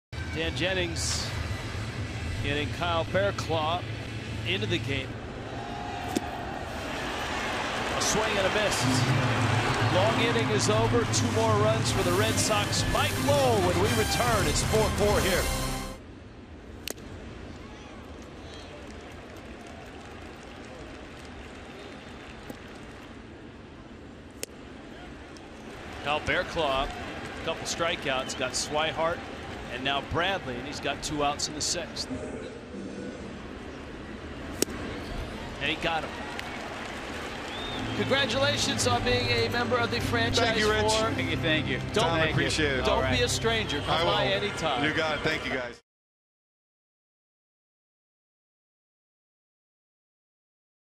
BOS@MIA: Barraclough fans four, earns first ML win
8/12/15: Kyle Barraclough strikes out four batters over 1 1/3 innings, earning the first win of his Major League career